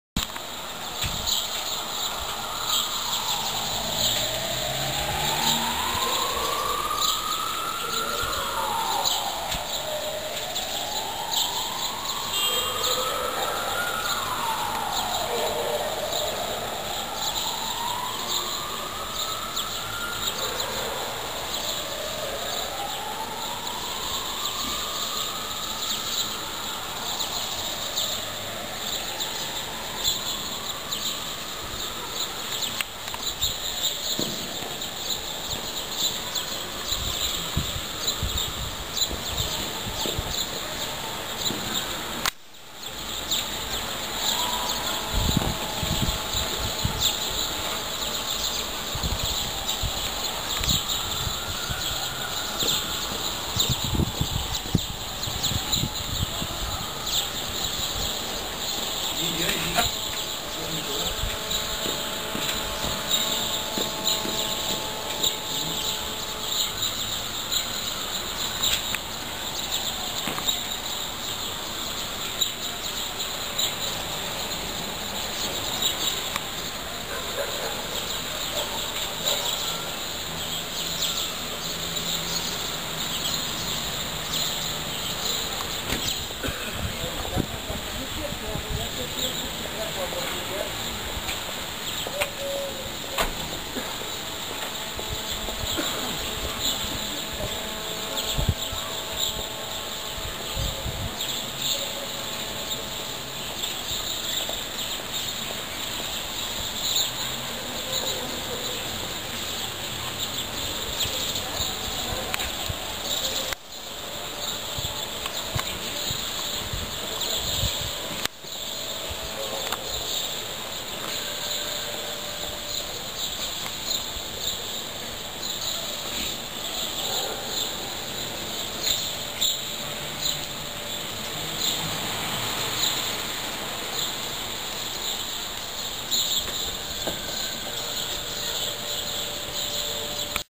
Norii din seara de 6 aprilie 2011
Norii de ploaie din seara de  6 aprilie, in jurul  orei  18.